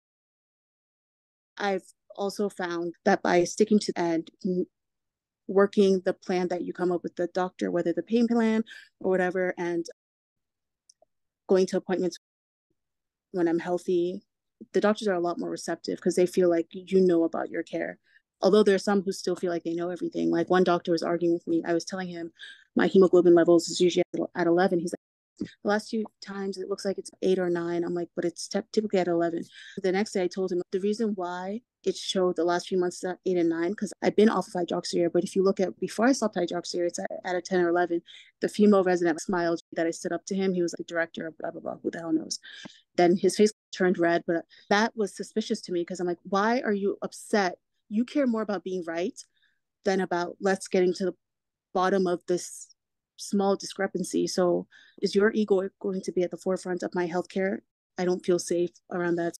Clip: Ego centric, patient centric. Balancing Motherhood, Community, Finances, and Sickle Cell
1 min watch. Fatima Muhammed-Ighile emphasizes the importance of patients actively participating in their healthcare, stating that doctors are generally more receptive when they see a patient's commitment. However, she shares an encounter with a doctor who prioritized his ego over her well-being, highlighting the challenges some patients face when advocating for themselves in medical settings.

Show notes 30 min read here: https:/